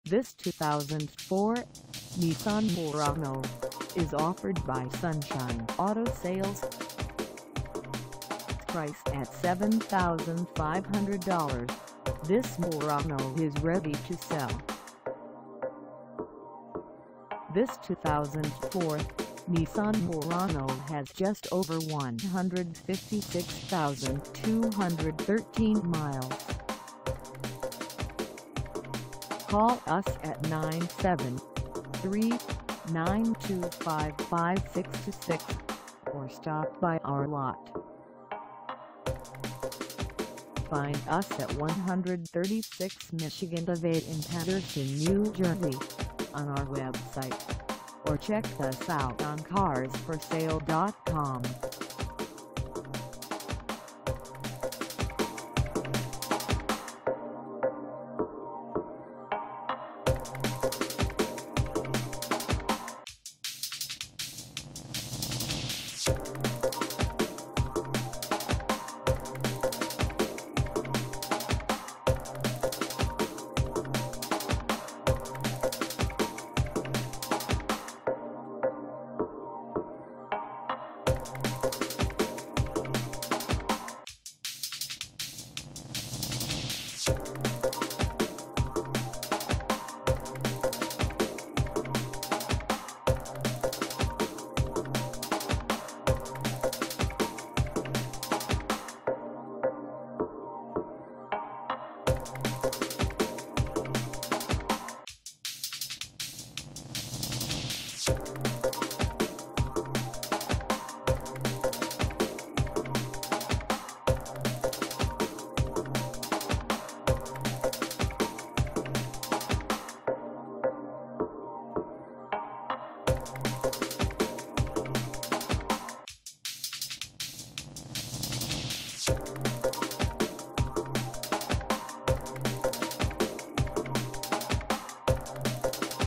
2004 Nissan Murano SE AWD NAVI 6 CD CHANGER HEATED SEATS FUL
This 2004 Nissan Murano SE AWD NAVI 6 CD CHANGER HEATED SEATS FULLY LOADED is for sale in Paterson, NJ 07503 at Sunshine Auto Sales.
Engine: V6 3.5L V6
Transmission: AUTOMATIC
Color: BURGANDY
Mileage: 156213
Contact Sunshine Auto Sales 136 Michigan Ave Paterson, NJ 07503 to test drive this 2004 Nissan Murano SE AWD NAVI 6 CD CHANGER HEATED SEATS FULLY LOADED today.